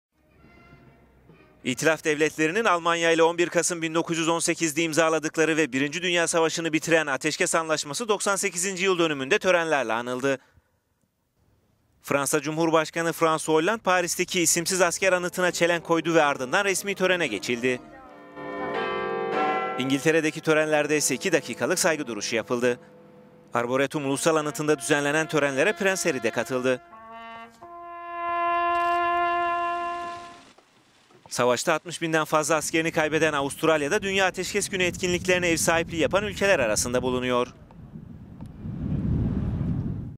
Birinci Dünya Savaşı'nı bitiren ateşkes 98. yıl dönümünde törenlerle anıldı
İtilaf devletleriyle Almanya arasında 11 Kasım 1918'de imzalanan  ve Birinci Dünya Savaşı'nı bitiren ateşkes anlaşması 98. yıl dönümünde törenlerle anıldı.

Fransa Cumhurbaşkanı François Hollande Paris'teki 'isimsiz Asker' anıtına çelenk koydu ev ardından resmi törene geçildi.

İngiltere'deki törenlerde ise iki dakikalık saygı duruşu yapıldı.

Arboretum Ulusal Anıtı'nda düzenlenen törenlere Prens Harry de katıldı.